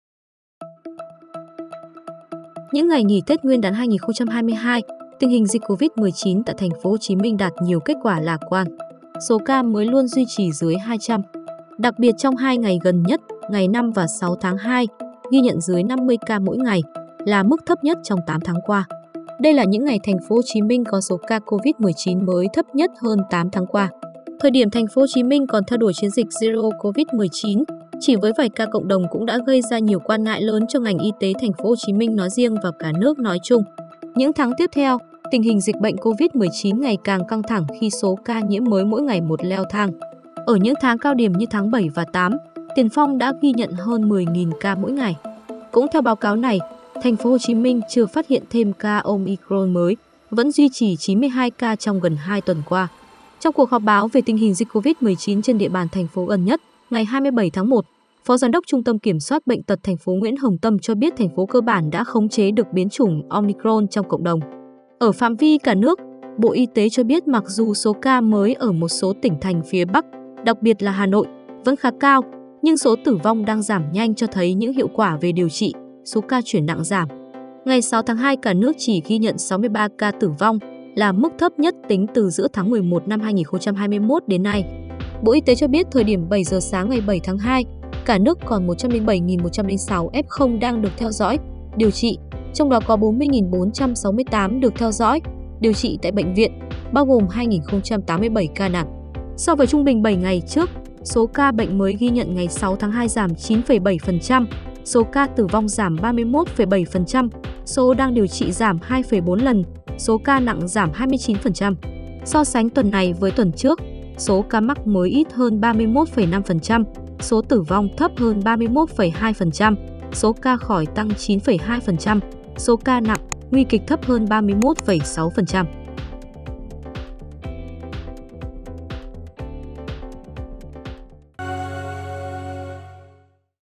Dịch COVID-19 đã được kiểm soát tốt, ca mới ở TP.HCM thấp nhất hơn 8 tháng qua
Những ngày nghỉ Tết Nguyên đán 2022, tình hình dịch COVID-19 tại TP.HCM đạt nhiều kết quả lạc quan. Số ca mới luôn duy trì dưới 200. Đặc biệt trong 2 ngày gần nhất (ngày 5 và 6-2), ghi nhận dưới 50 ca/ngày, là mức thấp nhất trong 8 tháng qua.